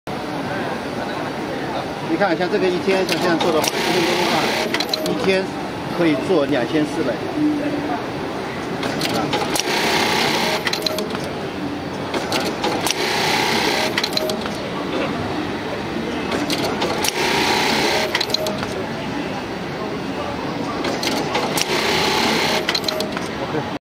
AUTOMATIC DOUBLE NEEDLE BELT LOOP SETTER, MODEL SP-254-ZD
HIGH SPEED DIRECT DRIVE (AC SERVO MOTOR) AUTOMATIC DOUBLE NEEDLE BELT LOOP SETTER SEWING MACHINE WITH  PROGRAMMABLE PANEL, FLASH CARD SUPPORTED, SEWING SPEED 2700RPM, LED LIGHT, BRAKE WHEEL, BEST QUALITY TABLE & STAND, POWER 220V, AIR PRESSURE 1.8 LT, COMPLETE SET  WITH STANDARD ACCESSORIES.